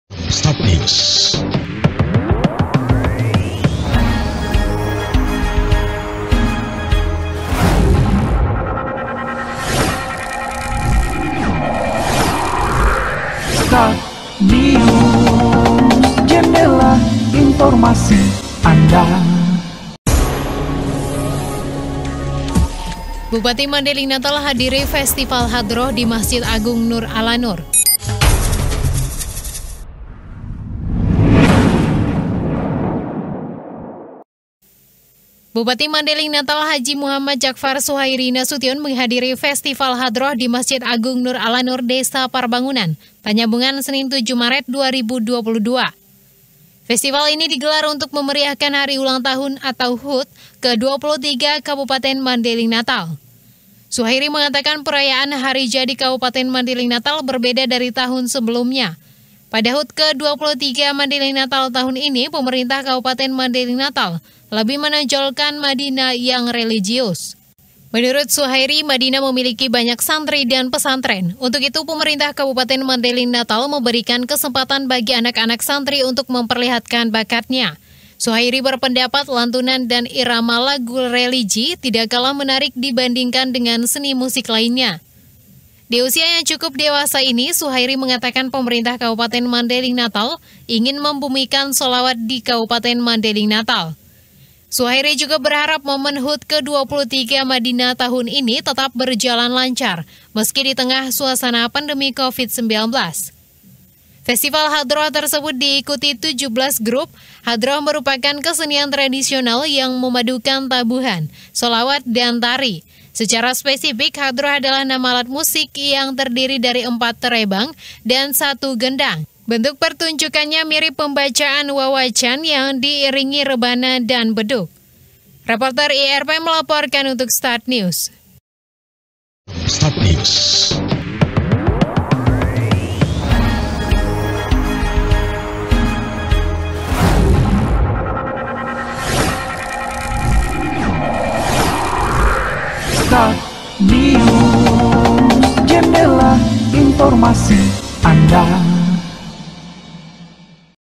Bupati Madina Hadiri Festival Hadroh di Masjid Agung Nur Ala Nur
Berita dan liputan informasi lengkap dari Bumi Gordang Sembilan Mandailing Natal